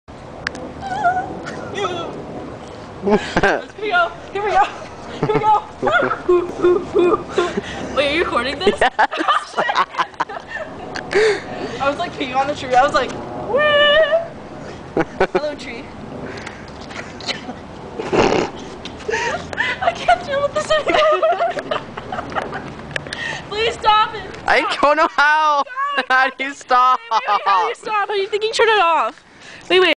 tree humping
Brogan and Ran molesting a tree.